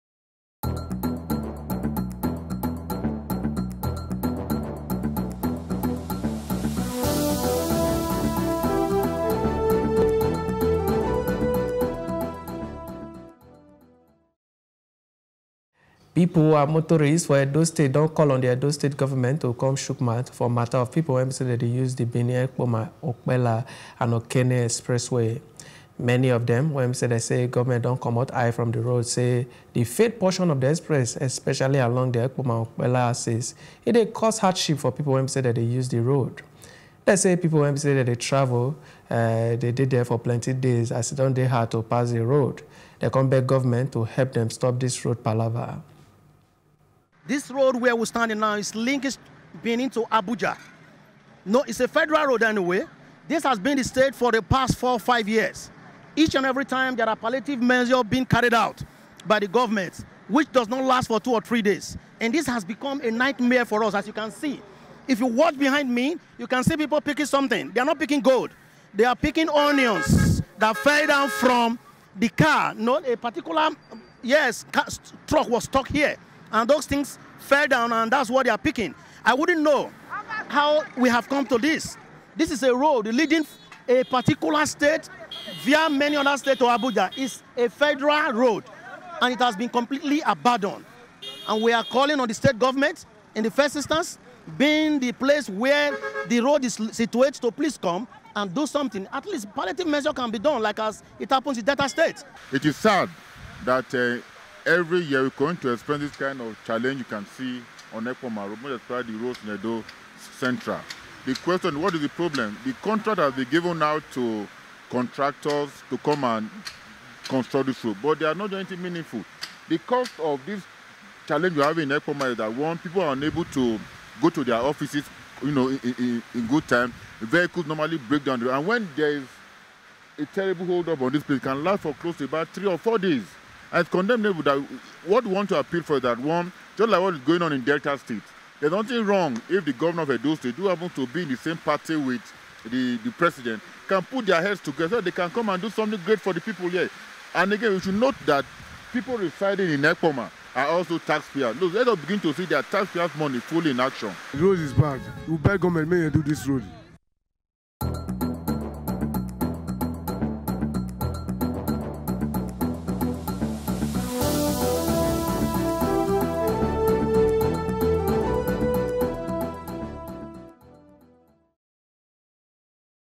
Motorists Dey Cry For Bad State Of Benin Expressway
Pepo and motorists for Edo state don call on the edo state government to come shook mouth for mata of pepo wen dem dey use the Benin-Ekpoma-Okpella and Okene expressway.

Many of them when dem dey say government’ don comot eye from the road say the failed portion of the expressway especially along with the Ekpoma-Okpella axis dey cause hardship for pepo wen dem dey use the road.